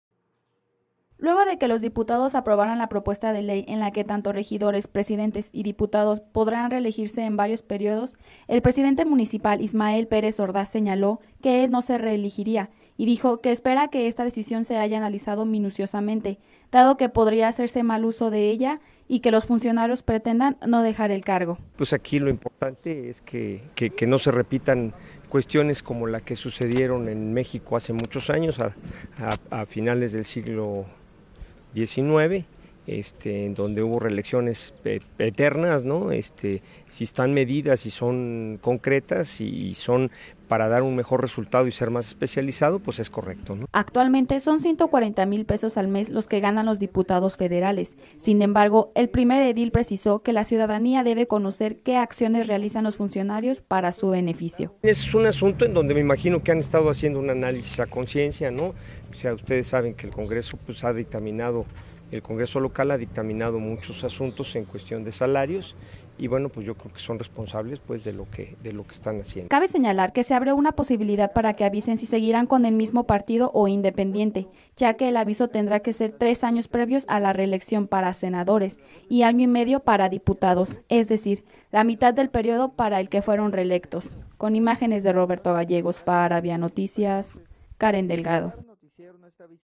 ISMAEL PÉREZ ORDAZ DIJO NO ESTAR CONVENCIDO CON LA REELECCIÓN DE PUESTOS
El Alcalde Ismael Pérez Ordaz dijo no estar convencido de la reelección de puestos dado que podría generarse lo que hace unos años, que no se quieran alejar de ellos, esto luego de que los Diputados aprobaron una iniciativa de propuesta para la reelección.